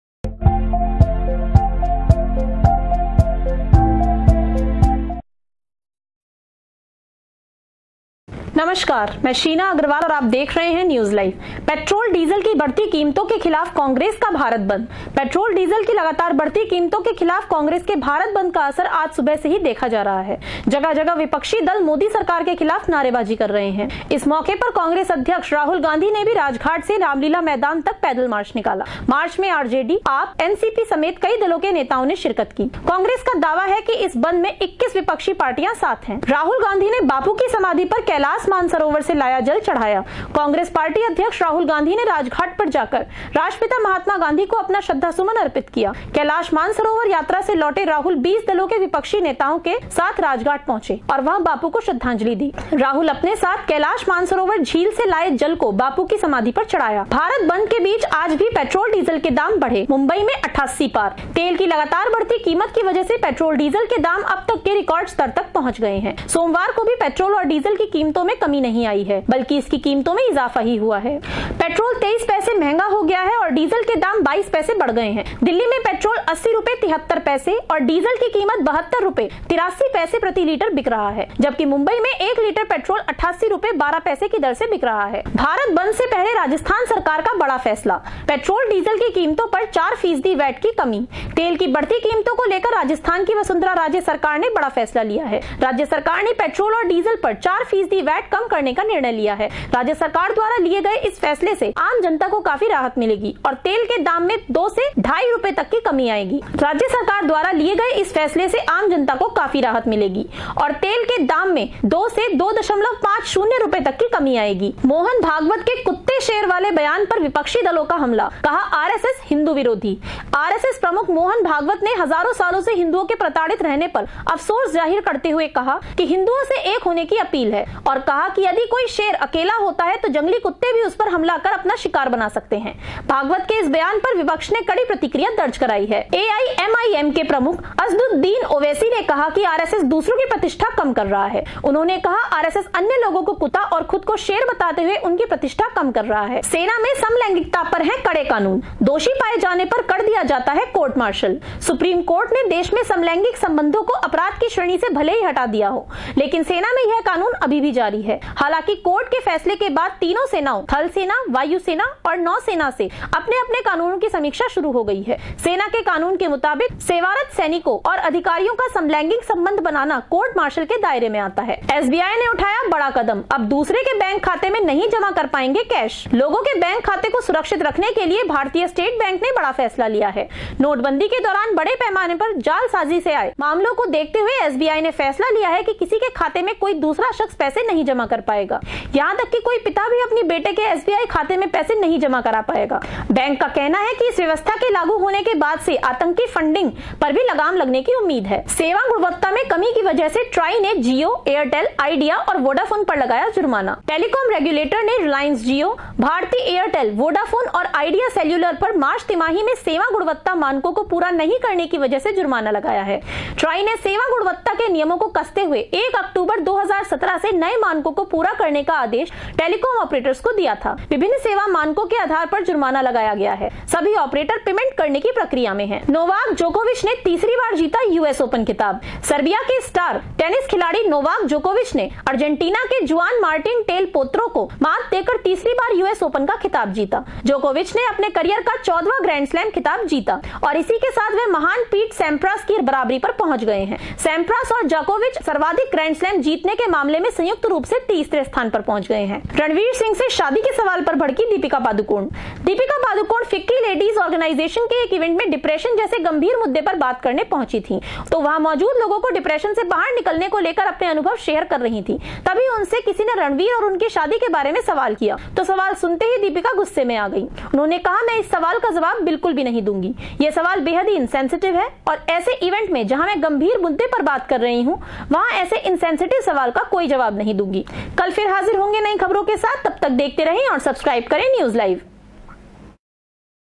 News Live 10-Sep-2018 || IIMT Media || रणवीर सिंह से शादी के सवाल पर भड़कीं दीपिका पादुकोण
पेट्रोल-डीजल की बढ़ती कीमतों के खिलाफ कांग्रेस का भारत बंद
पेट्रोल-डीजल की लगातार बढ़ती कीमतों के खिलाफ कांग्रेस के भारत बंद का असर आज सुबह से ही देखा जा रहा है. जगह-जगह विपक्षी दल मोदी सरकार के खिलाफ नारेबाजी कर रहे हैं. इस मौके पर कांग्रेस अध्यक्ष राहुल गांधी ने भी राजघाट से रामलीला मैदान तक पैदल मार्च निकाला. मार्च में आरजेडी, आप, एनसीपी समेत कई दलों के नेताओं ने शिरकत की. कांग्रेस का दावा है कि इस बंद में 21 विपक्षी पार्टियां साथ है.
राहुल गांधी ने बापू की समाधि पर कैलास मानसरोवर से लाया जल चढ़ाया
कांग्रेस पार्टी अध्यक्ष राहुल गांधी ने राजघाट पर जाकर राष्ट्रपिता महात्मा गांधी को अपना श्रद्धासुमन अर्पित किया। कैलास मानसरोवर यात्रा से लौटे  राहुल 20 दलों के विपक्षी नेताओं के साथ राजघाट पहुंचे और वहां बापू को श्रद्धांजलि दी। राहुल अपने साथ कैलास मानसरोवर झील से लाए जल को बापू की समाधि पर चढ़ाया।
भारत बंद के बीच आज भी पेट्रोल-डीजल के दाम बढ़े, मुंबई में 88 पार
तेल की लगातार बढ़ती कीमत की वजह से पेट्रोल-डीजल के दाम अब तक के रिकॉर्ड स्तर तक पहुंच गए हैं सोमवार को भी पेट्रोल और डीजल की कीमतों में कमी नहीं आई है बल्कि इसकी कीमतों में इजाफा ही हुआ है। पेट्रोल 23 पैसे महंगा हो गया है और डीजल के दाम 22 पैसे बढ़ गए हैं।  दिल्ली में पेट्रोल 80 रुपये 73 पैसे  और  डीजल की कीमत 72 रुपये 83 पैसे प्रति लीटर बिक रहा है। जबकि मुंबई में एक लीटर पेट्रोल 88 रुपये 12 पैसे की दर से बिक रहा है
भारत बंद से पहले राजस्थान सरकार का बड़ा फैसला, पेट्रोल-डीजल की कीमतों पर 4 फीसदी वैट की कमी
तेल की बढ़ती कीमतों को लेकर राजस्थान की वसुंधरा राजे सरकार ने बड़ा फैसला लिया है।  राज्य सरकार ने पेट्रोल और डीजल पर 4 फीसदी वैट कम करने का निर्णय लिया है। राज्य सरकार द्वारा लिए गए इस फैसले से आम जनता को काफी राहत मिलेगी और तेल के दाम में 2 से 2.50 रुपये तक की कमी आएगी।

मोहन भागवत के 'कुत्ते-शेर' वाले बयान पर विपक्षी दलों का हमला, कहा- RSS हिन्दू विरोधी
RSS प्रमुख मोहन भागवत ने हजारों सालों से हिंदुओं के प्रताड़ित रहने पर अफसोस जाहिर करते हुए हिंदुओं से एक होने की अपील की और कहा कि ‘यदि कोई शेर अकेला होता है, तो जंगली कुत्ते भी उस पर हमला कर अपना शिकार बना सकते हैं.’ भागवत के इस बयान पर विपक्ष ने कड़ी प्रतिक्रिया दर्ज कराई है. एआईएमआईएम के प्रमुख असदुद्दीन ओवैसी ने कहा कि RSS दूसरों की प्रतिष्ठा कम कर रहा है. उन्होंने कहा, 'RSS अन्य लोगों को कुत्ता और खुद को शेर बताते हुए, उनकी प्रतिष्ठा कम कर रहा है.
सेना में समलैंगिकता पर हैं कड़े कानून, दोषी पाए जाने पर कर दिया जाता है कोर्ट मार्शल
सुप्रीम कोर्ट ने देश में समलैंगिक संबंधों को अपराध की श्रेणी से भले हटा दिया हो, लेकिन सेना में यह कानून अभी भी जारी है। हालांकि कोर्ट के फैसले के बाद तीनों सेनाओं थल सेना, वायु सेना और नौसेना में अपने-अपने कानूनों की समीक्षा शुरू हो गई है।  सेना के कानून के मुताबिक, सेवारत सैनिकों और अधिकारियों का समलैंगिक संबंध बनाना कोर्ट मार्शल के दायरे में आता है।

SBI ने उठाया बड़ा कदम, अब दूसरे के बैंक खाते में नहीं जमा कर पाएंगे कैश
लोगों के बैंक खाते को सुरक्षित रखने के लिए भारतीय स्टेट बैंक (SBI) ने बड़ा फैसला लिया है. नोटबंदी के दौरान बड़े पैमाने पर जालसाजी के आए मामले को देखते हुए एसबीआई ने फैसला लिया है कि किसी के खाते में कोई दूसरा शख्स पैसे नहीं जमा करा पाएगा. यहां तक की कोई पिता भी अपने बेटे के SBI खाते में पैसे नहीं जमा करा पाएगा. बैंक का कहना है कि इस व्यवस्था के लागू होने के बाद से आतंकी फंडिंग पर भी लगाम लगने की उम्मीद है.

सेवा गुणवत्ता में कमी की वजह से TRAI ने जियो, एयरटेल, आइडिया और वोडाफोन पर लगाया जुर्माना
टेलिकॉम रेग्युलेटर ने रिलायंस जियो, भारती एयरटेल, वोडाफोन और आइडिया सेल्युलर पर मार्च तिमाही में सेवा गुणवत्ता मानकों को पूरा नहीं करने की वजह से जुर्माना लगाया है। TRAI ने सेवा गुणवत्ता के नियमों को कसते हुए 1 अक्टूबर 2017 से नए मानकों को पूरा करने का आदेश टेलिकॉम ऑपरेटर्स को दिया था। विभिन्न सेवा मानकों के आधार पर जुर्माना लगाया गया है। सभी ऑपरेटर पेमेंट करने की प्रक्रिया में हैं।
नोवाक जोकोविच ने तीसरी बार जीता यूएस ओपन खिताब
सर्बिया के स्टार टेनिस खिलाड़ी नोवाक जोकोविच ने अर्जेंटीना के जुआन मार्टिन डेल पोत्रो को मात देकर तीसरी बार यूएस ओपन का खिताब जीता। जोकोविच ने अपने करियर का 14वां ग्रैंडस्लेम खिताब जीता और इसी के साथ वह महान पीट सैमप्रास की बराबरी पर पहुंच गए हैं।  सैमप्रास और जोकोविच सर्वाधिक ग्रैंडस्लैम खिताब जीतने के मामले में संयुक्त रूप से तीसरे स्थान पर पहुंच गए हैं।

रणवीर सिंह से शादी के सवाल पर भड़कीं दीपिका पादुकोण
दीपिका पादुकोण फिक्की लेडिज ऑर्गेनाइजेशन के एक इवेंट में डिप्रेशन जैसे गंभीर मुद्दे पर बात करने पहुंची थीं. वो वहां मौजूद लोगों को डिप्रेशन से बाहर निकलने को लेकर अपने अनुभव शेयर कर रही थीं तभी उनसे किसी ने रणवीर और उनकी शादी के बारे में सवाल किया तो सवाल सुनते ही दीपिका गुस्से में आ गईं. उन्होंने कहा कि मैं इस सवाल का जवाब बिलकुल भी नहीं दूंगी. ये सवाल बेहद ही इनसेंसटिव है. और ऐसे इवेंट में, जहां मैं गंभीर मुद्दे पर बात कर रही हूं वहां ऐसे इनसेंसटिव सवाल का जवाब नहीं दूंगी.